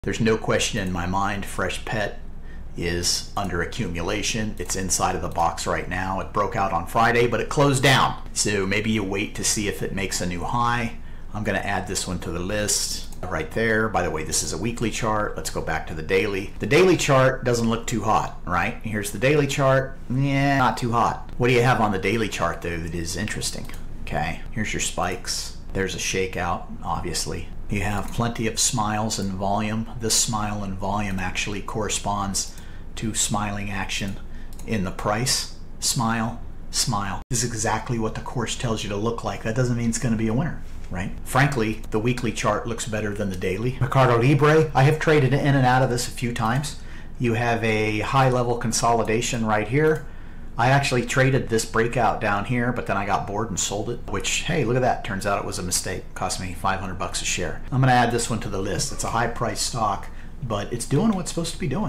Mastering Stock Charts: Daily vs Weekly Insights!
Join us as we dissect the Fresh Pet stock performance through daily and weekly chart comparisons. Our expert explores accumulation patterns, trading insights, and personal experiences. Don't miss this essential guide for stock analysis enthusiasts looking to enhance their trading strategies!